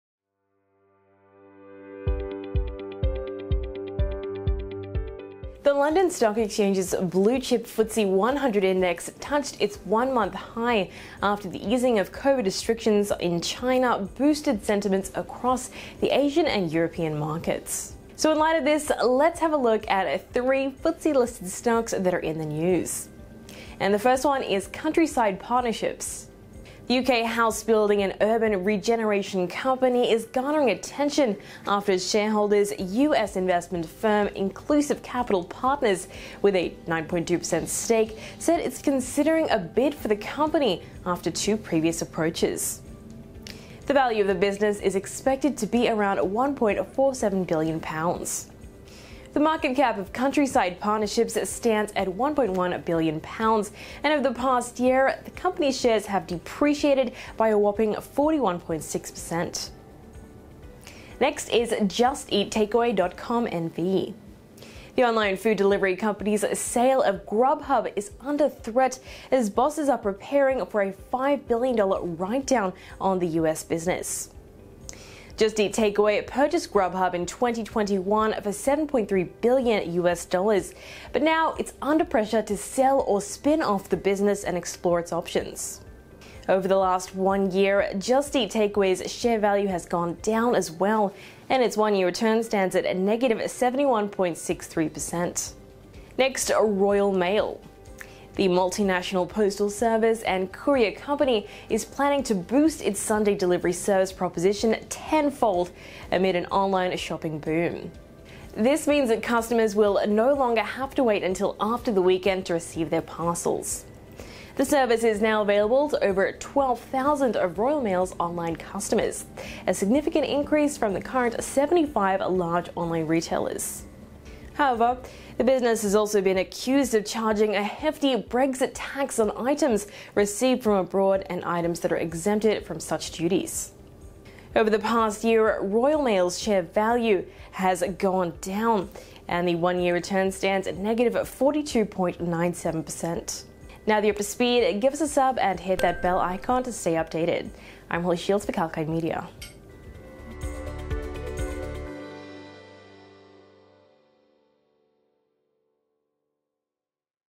CSP, JET, RMG: Why are these stocks in news?
The London Stock Exchange’s bluechip FTSE 100 index touched its one-month high after easing of Covid restrictions in China boosted sentiments across Asian and European markets. Let us look at 3 FTSE-listed stocks that are in the news in the video by Kalkine Media.